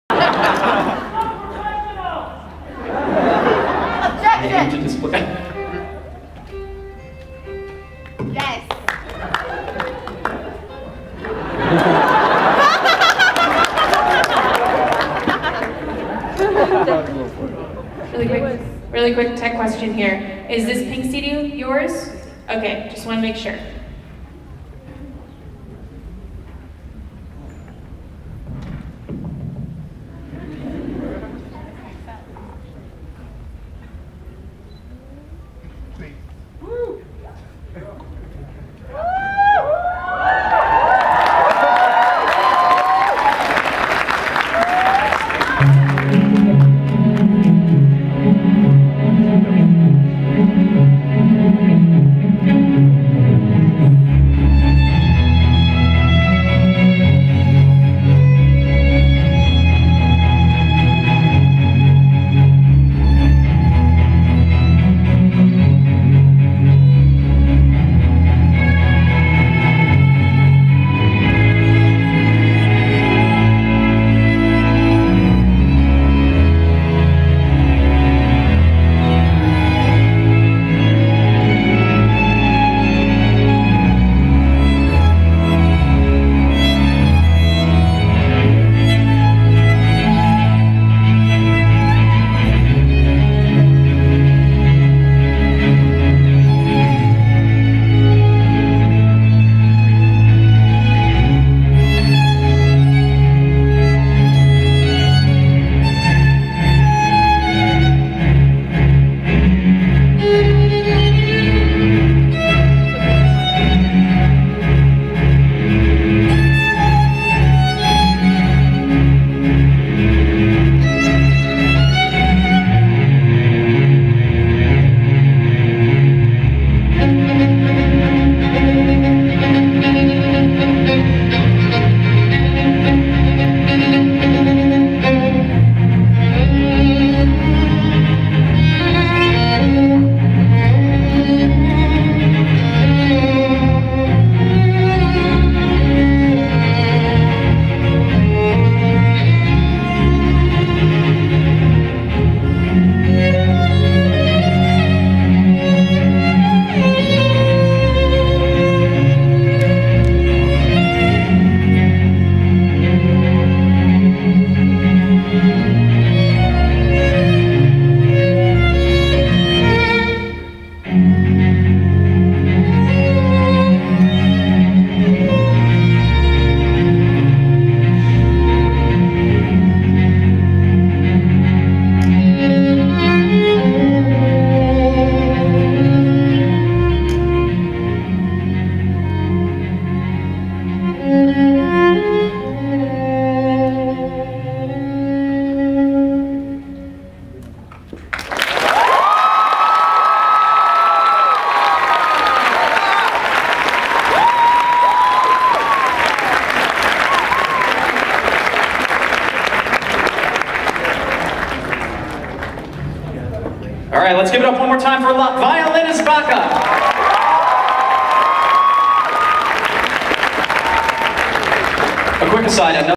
Violinist at Chibi Masquerade Cosplay
Anime Expo 2012 LA Convention. Violinist plays live at the Chibi Masquerade